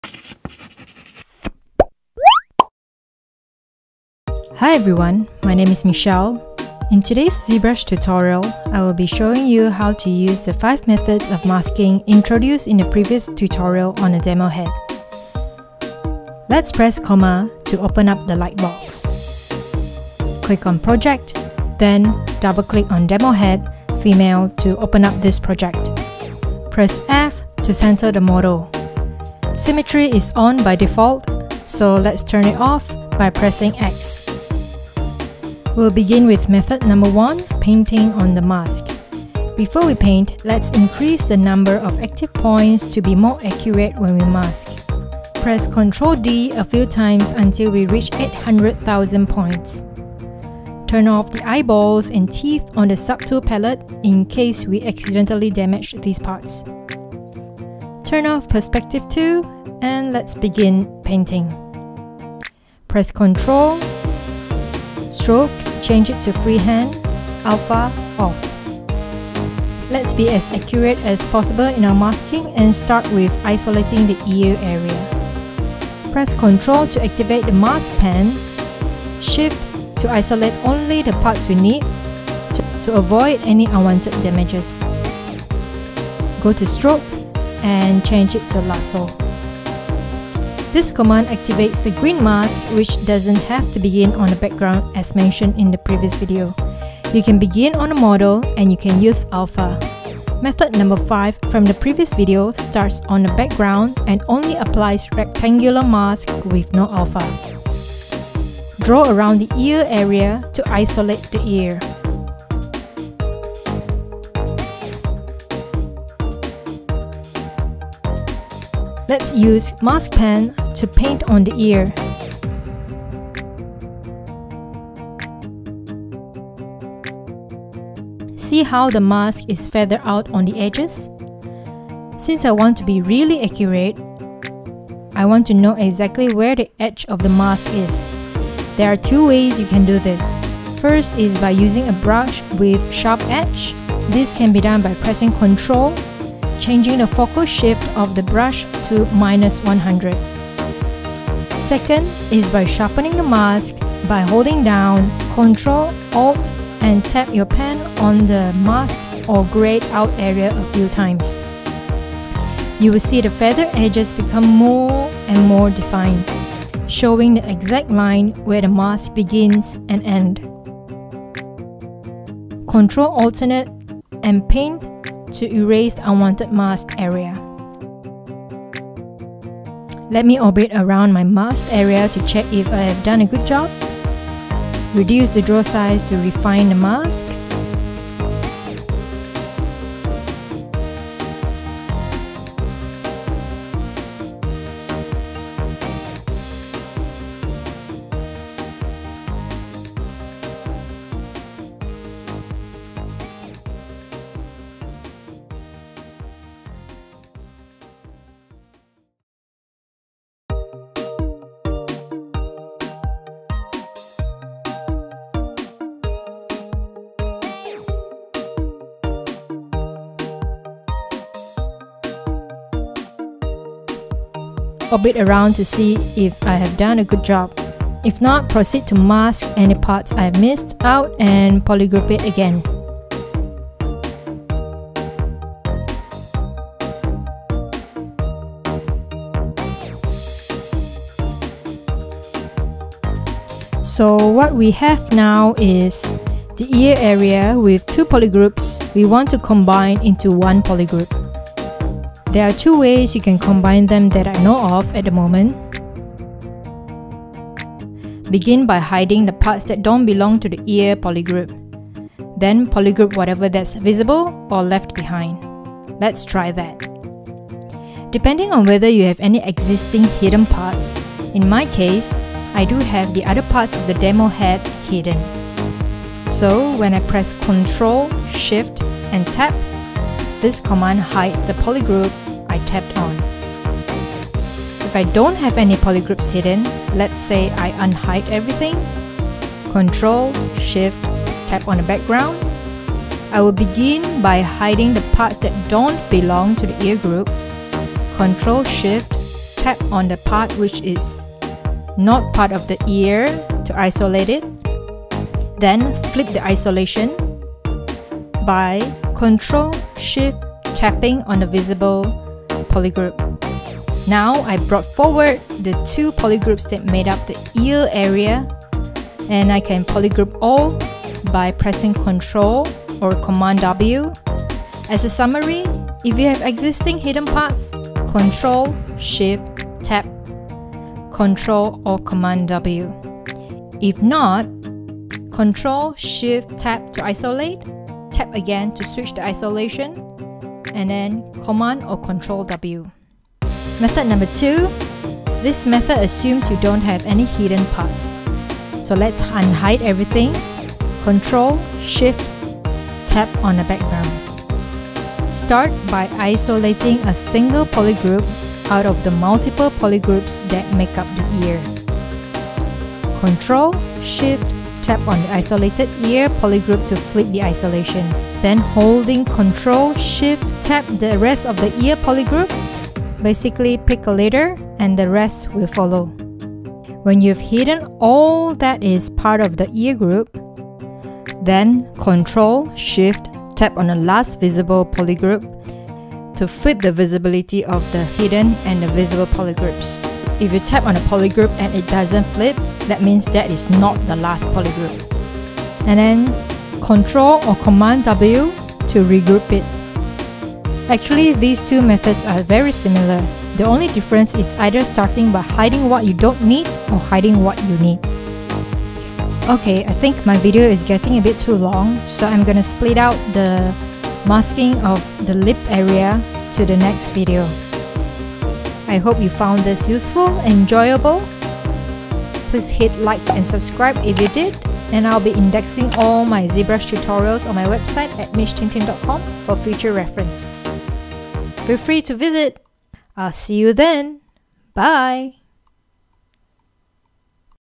ZB_05 | 2 ways to combine masked area with multiple polygroups.
for a more indexed tutorials sorted according to categories and tags.

00:00 Intro
00:17 Open project
00:45 Increase active points
01:15 Isolate ear area
02:00 Using Method 1 to mask
02:20 Use Sharp edged mask
02:30 Sharpen mask
02:54 Erase mask
03:35 Polygroup masked area
03:51 Polygroup missed area
04:13 Combine polygroups 2 ways
04:22 Method 1: Existing hidden polygroups
04:45 Method 1: No hidden polygroups
05:20 Polygroup all
05:25 Summary of method 1
05:45 Method 2: No hidden polygroups
05:51 Unhide everything
06:05 Pick a leader, the rest will follow
06:50 to be continued.
07:06 Hit like and subscribe..